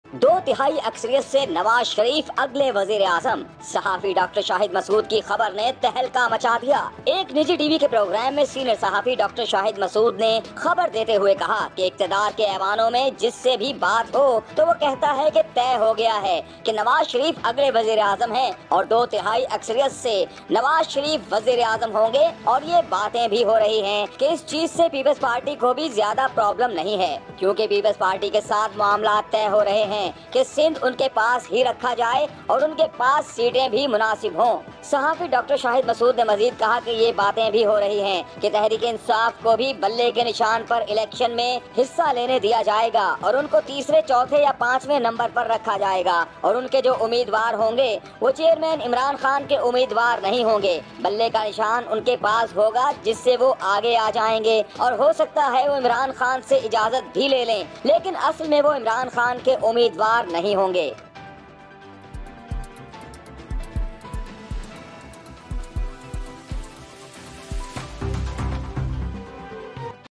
Who will be the Next Prime Minister , Imran Khan or Nawaz Sharif? | Dr Shahid Masood News | PM News
اگلا وزیراعظم کون ہوگا، عمران خان یا نواز شریف؟ | ڈاکٹر شاہد مسعود نیوز | وزیر اعظم نیوز

If you want to be a part of my video send your video saying, "Hi my name is (your name), I'm from (your city) " to